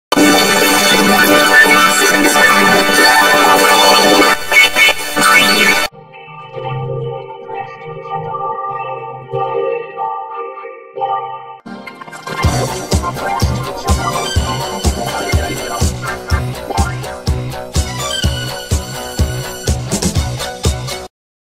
3 New Leafyormulators
Leafyormulator V68:
Green Out + Fun House + Fun House (Orientaion: Vertical)
Layer: 119th image of Leafy
Audio: Mangle^2 + Fmaj Vocoder + Pitch +9

Leafyormulator V69:
Video: 763ormulator V9 but gradient map is LeafyFlangedSawChorded
Layer: 765th image of Leafy (Full Fade)
Audio: ERZATZ 013 + Full Chord

Leafyormulator V70:
Video: LeafyFlangedSawChorded + Sicario Major 222
Layer: 56th image of Leafy (Full Fade)
Audio:
Track 1: Full Chord
Track 2: TDVM953 Pixitracker Major 19